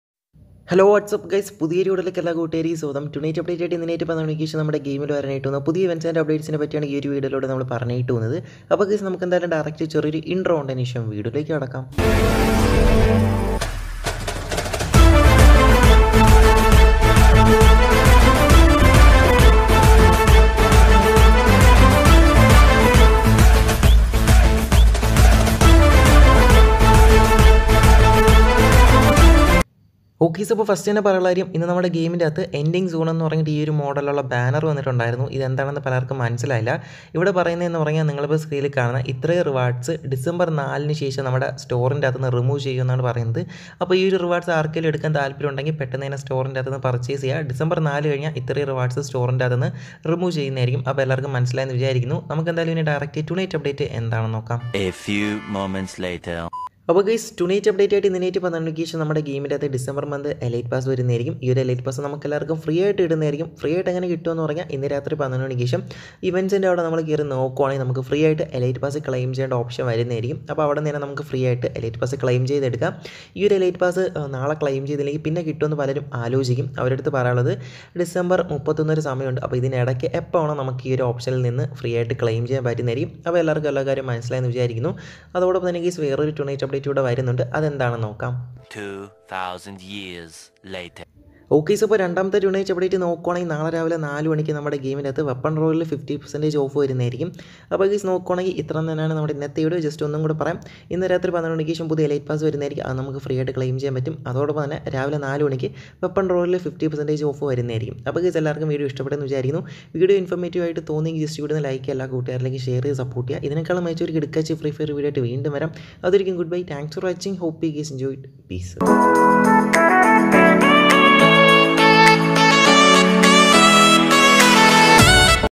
TONIGHT UPDATE OF FREE FIRE MALAYALAM 🔥| FREE ELITE PASS + DISCOUNT EVENT 🤯| FF TONIGHT UPDATE
FREE FIRE TONIGHT UPDATE | 1st DECEMBER NEW EVENT IN FREE FIRE MALAYALAM

👇More Details

This video is just for entertainment and Education purpose Only.